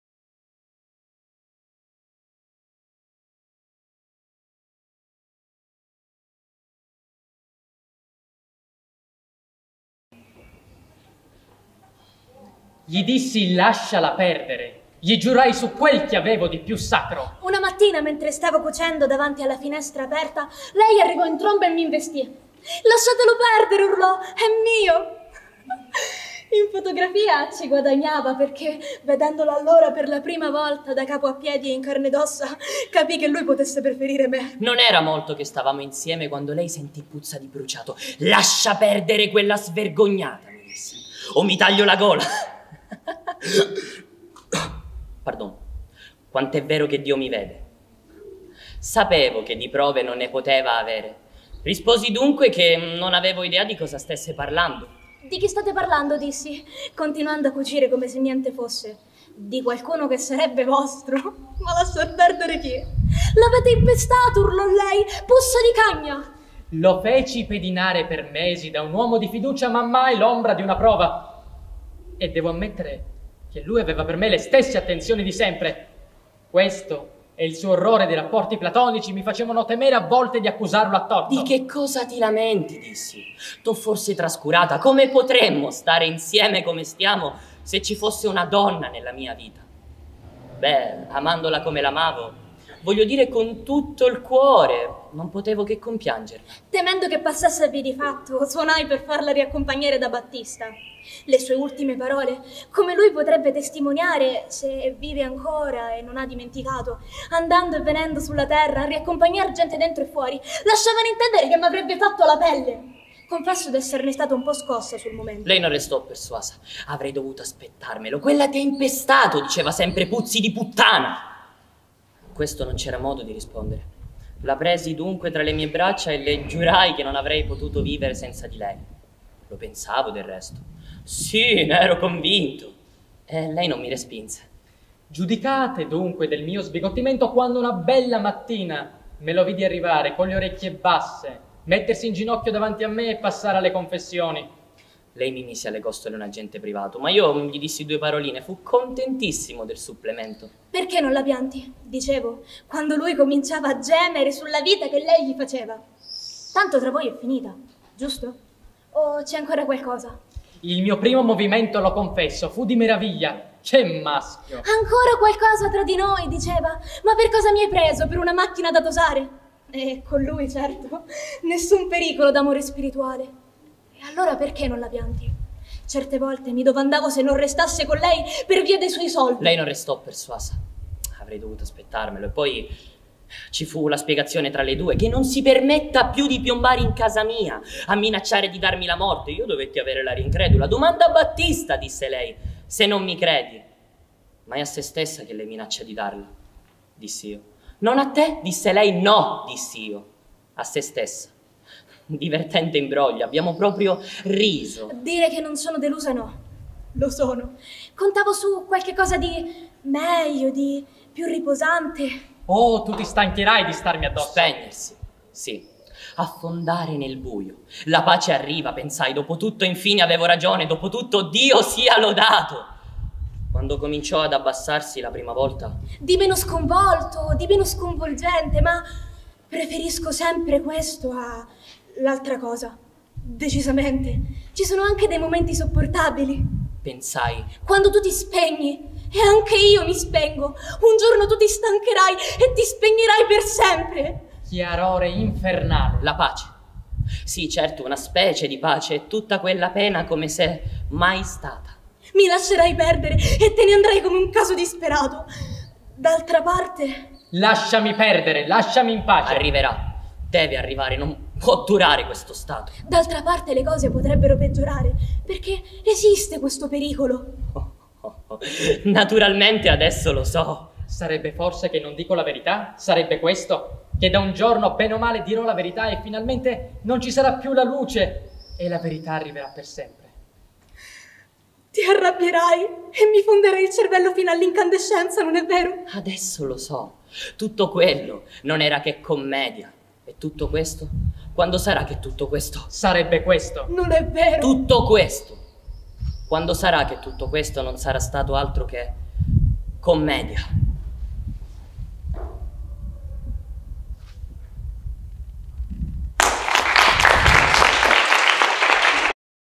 Accademia, sezione Balestra: studio su Commedia di Samuel Beckett
Studio degli allievi dell'Accademia d'Arte del Dramma Antico su Commedia di Samuel Beckett.
Nel video i ragazzi della Masterclass (allievi dai 9 ai 21 anni)